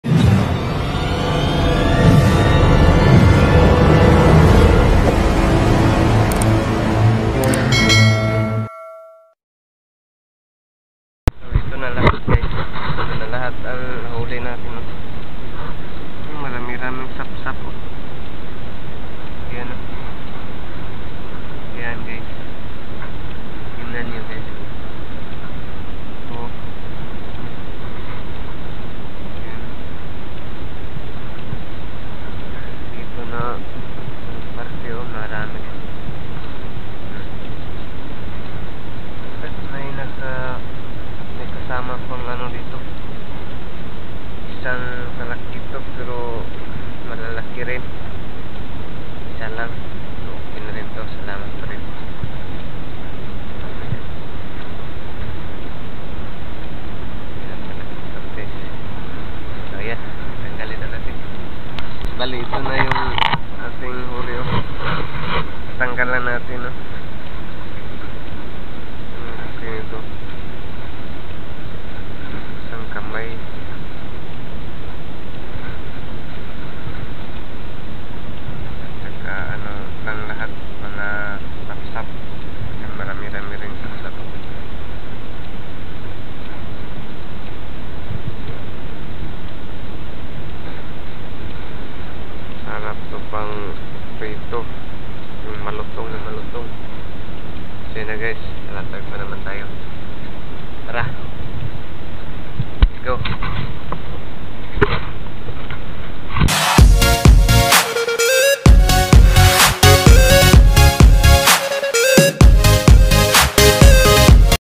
Daming sapsap at may malaking talakitok ang nadali sa aking lambat.
Nanghuhuli ako ng isda dito sa aming lugar gamit ko ay lambat at ang daming sapsap at may malaking talakitok ang nadali sa aking lambat